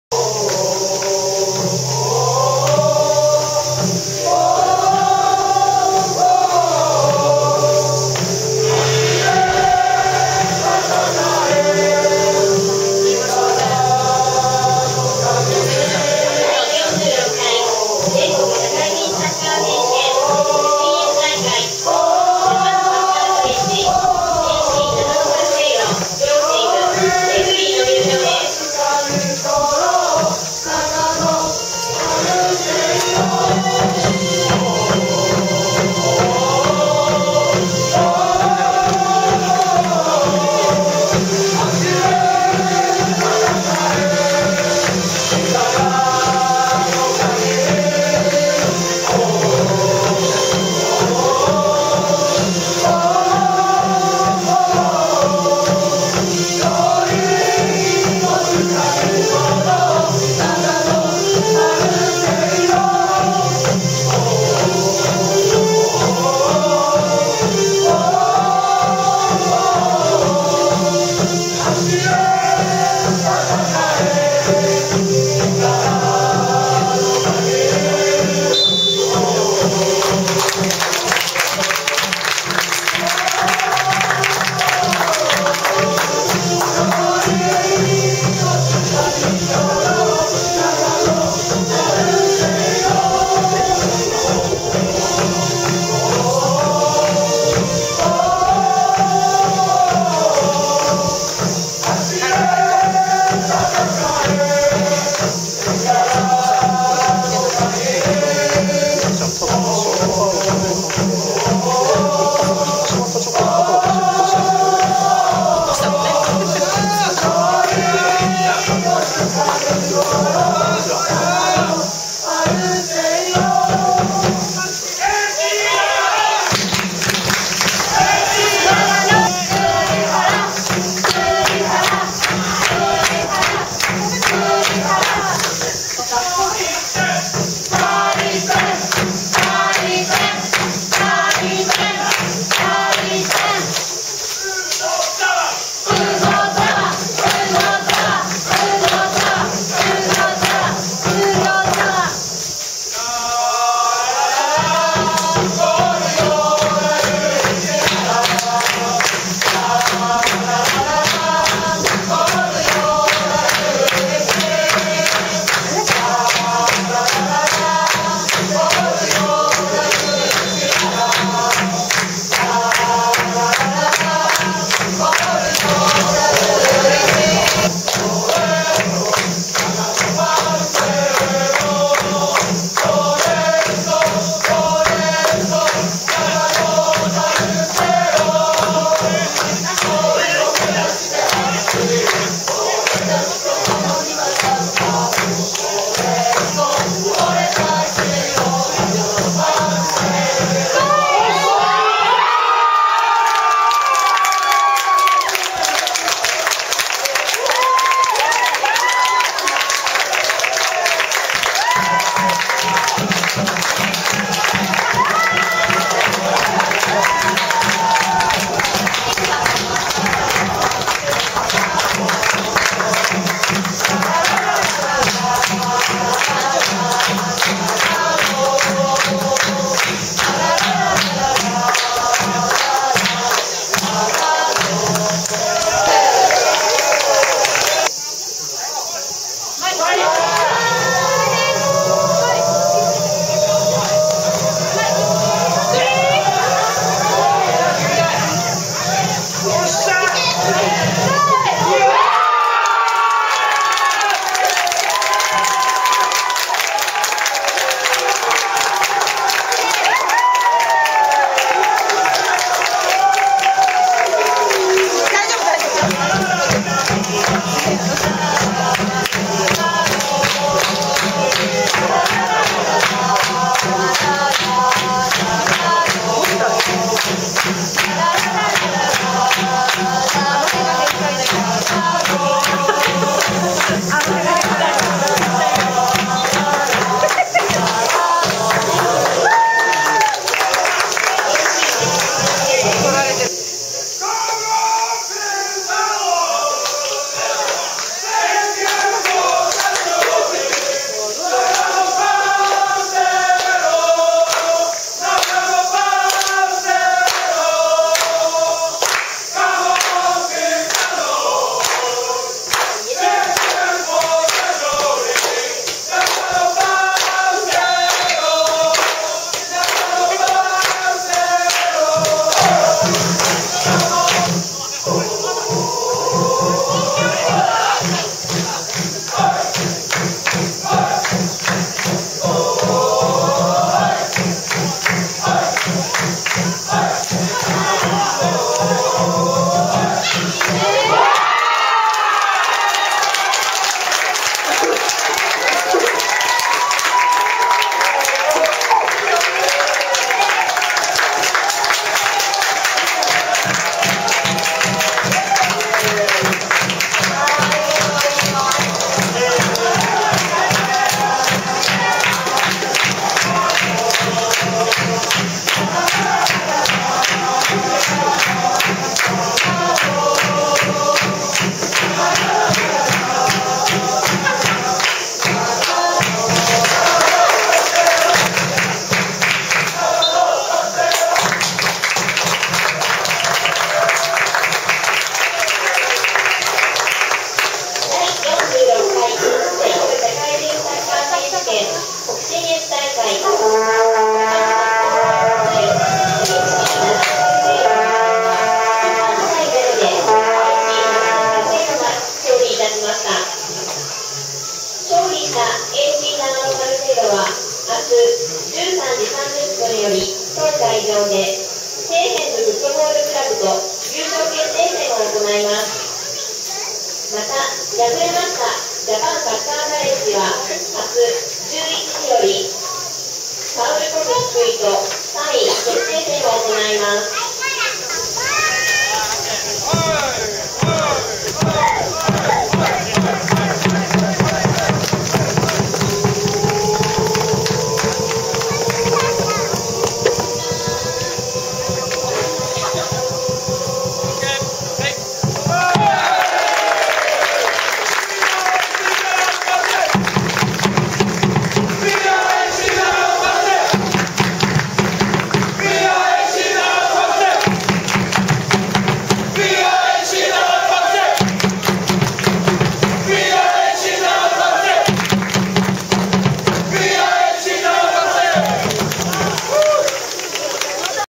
2010/8/14 JAPANサッカーカレッジ vs AC長野パルセイロ
第46回全国社会人サッカー選手権大会 北信越大会 準決勝
2010/8/14 JAPANサッカーカレッジ vs AC長野パルセイロ

AC長野パルセイロ 得点シーンダイジェスト